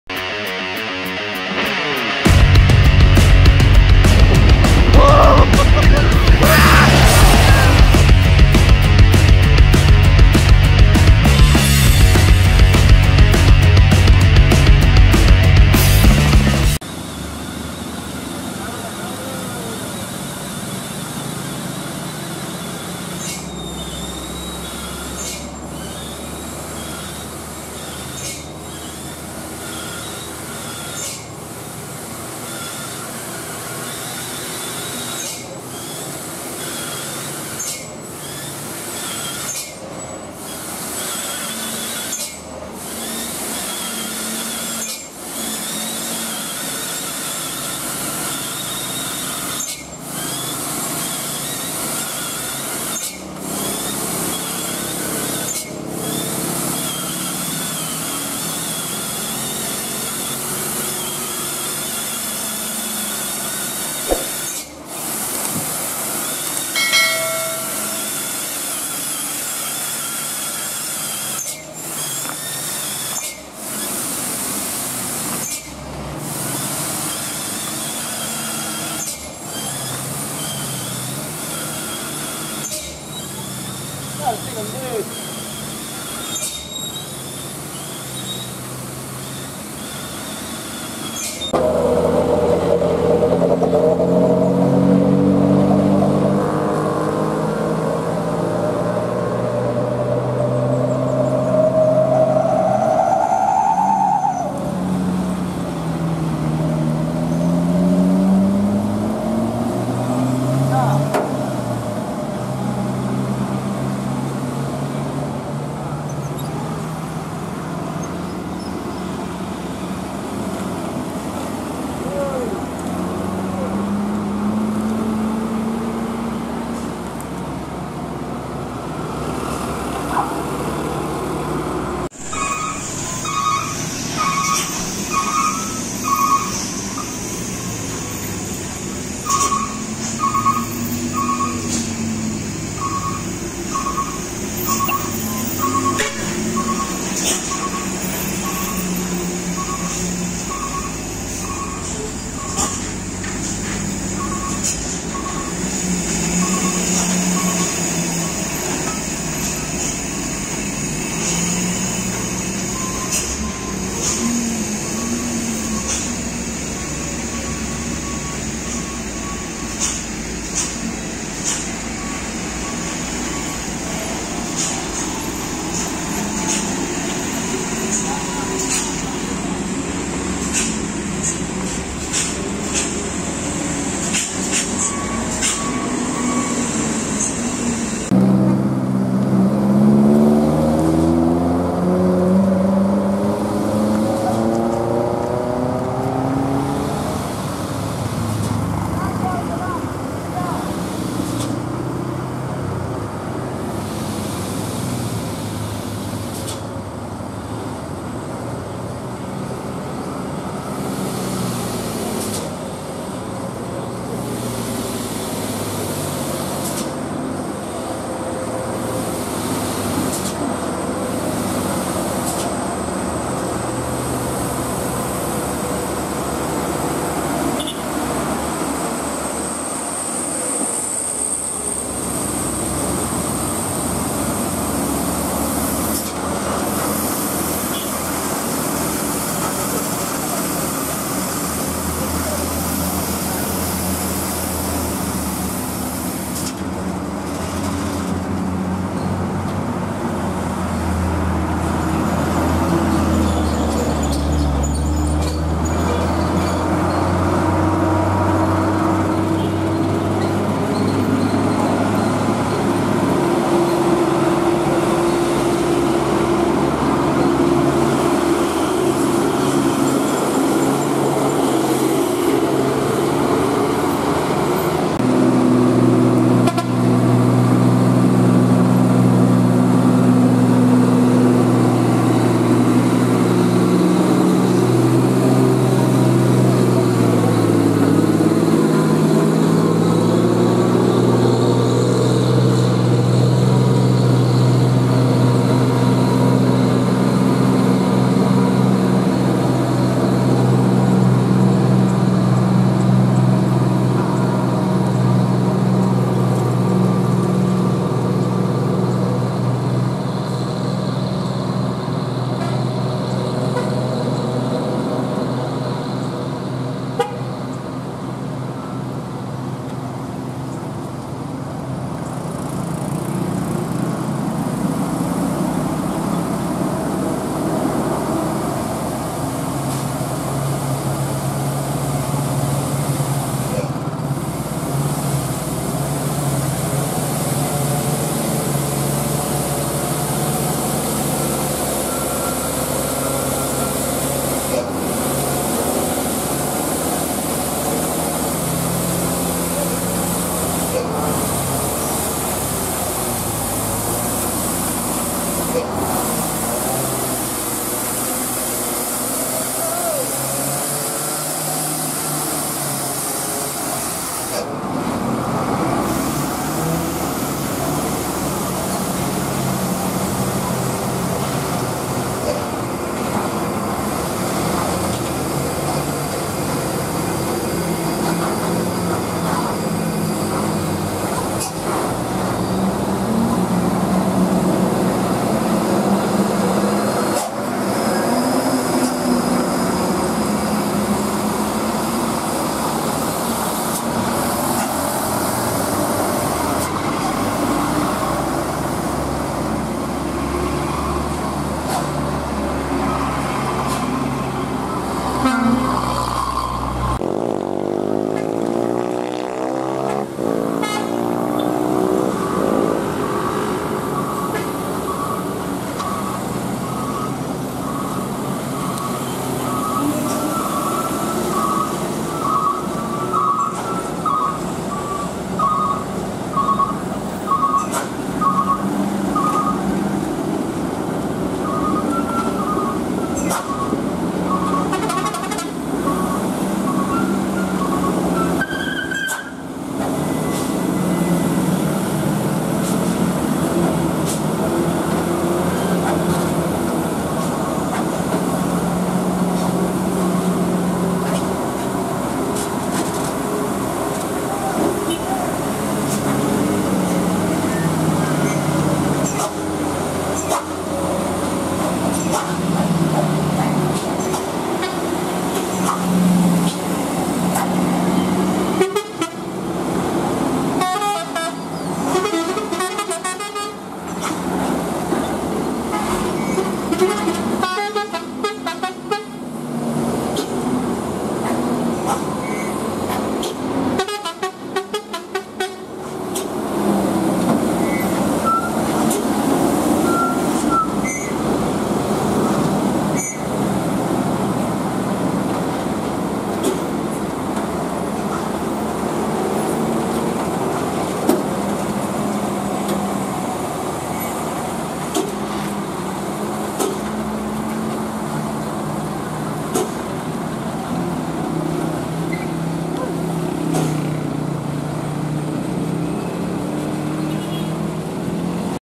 Sopir Truk Pemberani !!! Tak Takut Terbalik Bawa Truk Muatan Sangat Tinggi Turuni Sitinjau Lauik
komentar nya dan shared video ini jika bermanfaat untuk orang lain
follow juga